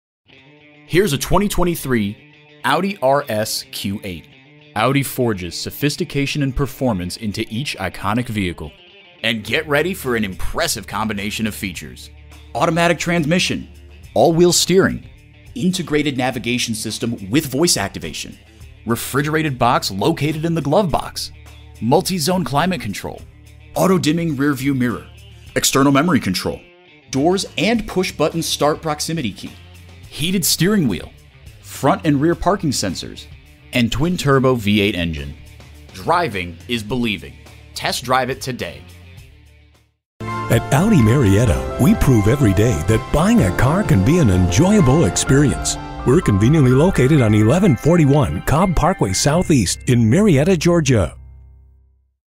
New 2023 Audi RS Q8 Marietta Atlanta, GA U56607 - SOLD
Year: 2023
Make: Audi
Model: RS Q8
Trim: 4.0T
Engine: 4.0L V8 Turbocharged DOHC 32V LEV3-ULEV125 591hp
Transmission: 8-Speed Automatic with Tiptronic
Color: Black
Interior: Ar
Stock #: U56607
VIN: WU1ARBF13PD034865

Address:
1715 Cobb Parkway South